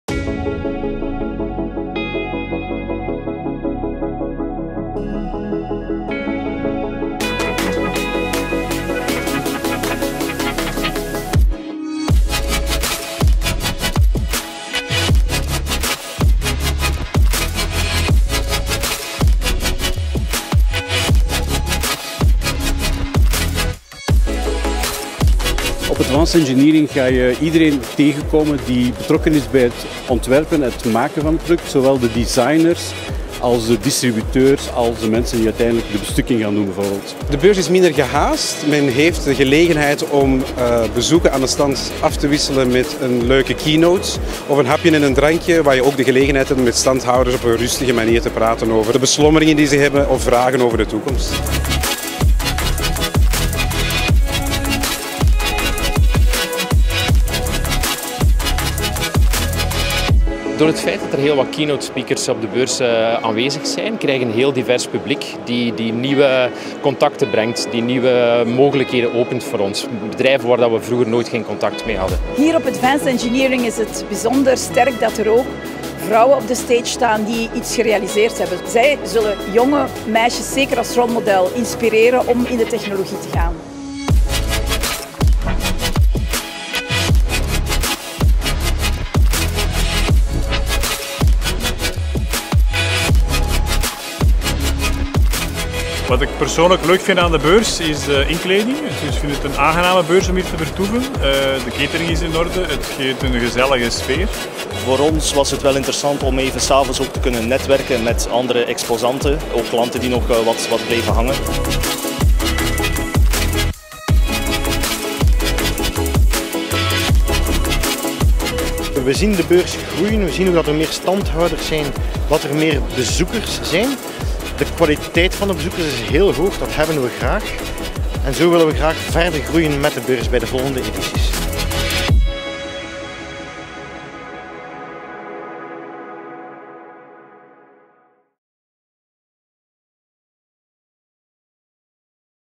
Advanced Engineering 2019 - Aftermovie
Advanced Engineering is een innovatief event over de creatie en de productie van vernieuwende producten. U vindt er alles terug om uw producten klaar te maken voor morgen. Alle aspecten komen aan bod: design, engineeringsproces en prototyping, materialen en componenten, alsook elektronica en connectiviteit.
Dit event is het ideale platform om uw nieuwe technologieën, producten en innovaties te tonen aan de juiste profielen over sectoren heen.

Enkele cijfers van de editie op 22 & 23 mei 2019 in Flanders Expo Gent:

- 1.764 personen (+42%) bezochten Advanced Engineering.
- Hiervan kwamen 211 professionele bezoekers door de poorten van het Techniekfestival.
- 873 bezoekers bezochten een conferentie
- 54 geïnteresseerden namen deel aan de innovation tours en bezochten zo de finalisten voor de innovation award